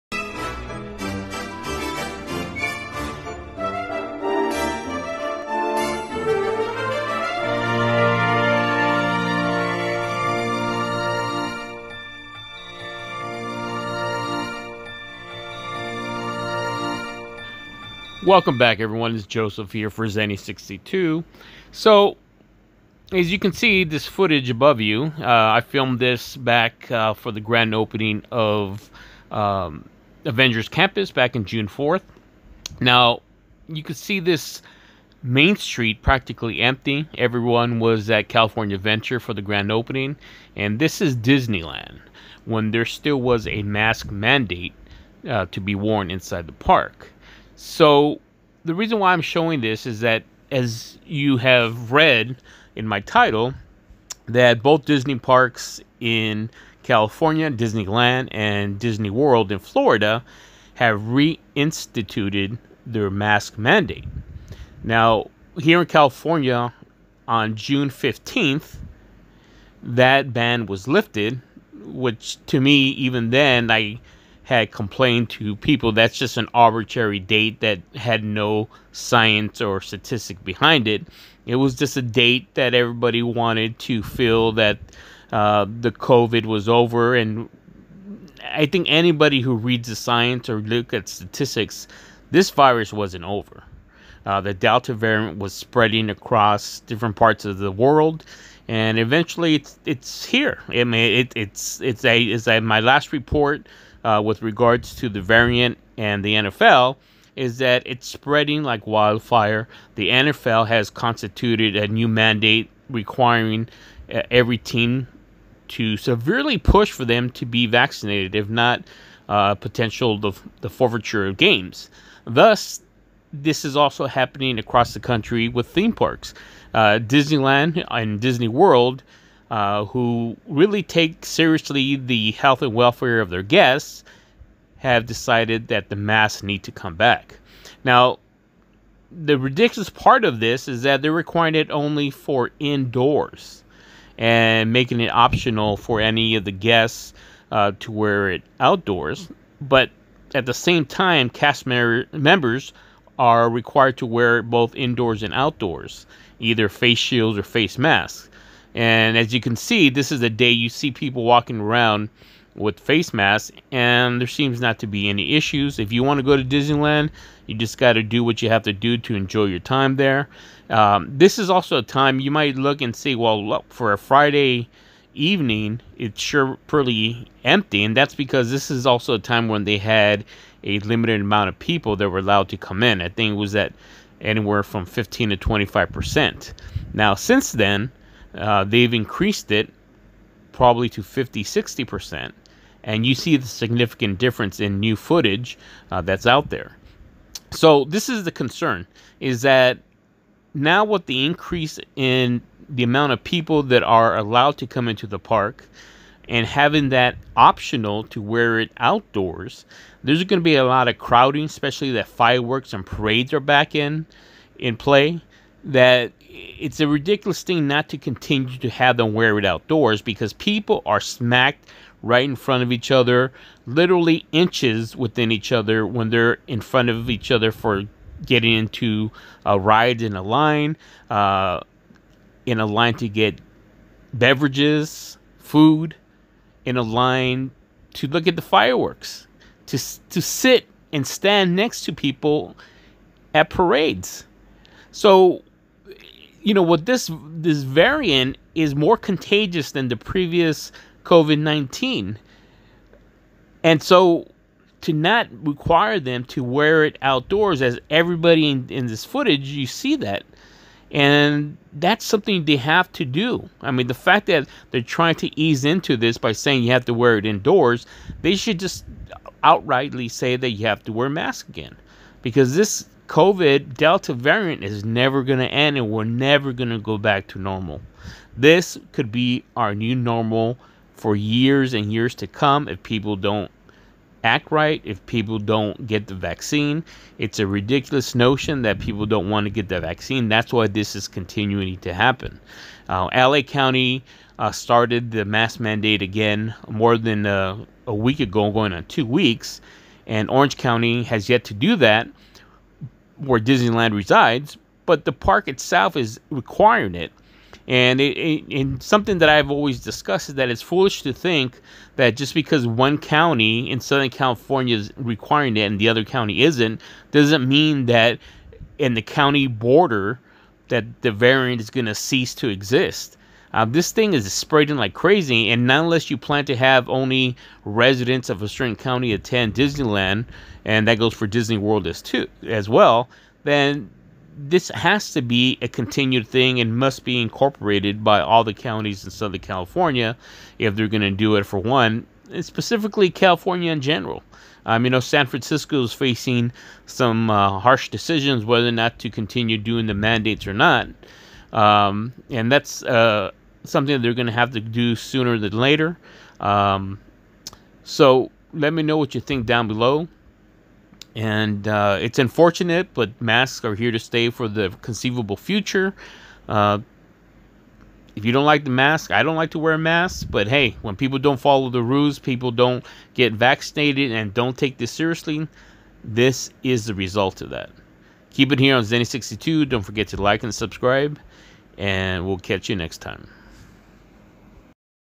Disney Parks Brings Back Mask Mandate Due To Delta Variant Surge By Joseph Armendariz
it is not surprising due to the surge in the Delta variant that theme parks across the country are reconsidering to bring back the mask mandate.
 Disney Parks is the first to reinstitute it's mask mandate to all there theme parks in the United States.

Disneyland footage courtesy of Joseph Armendariz
photo courtesy of deadline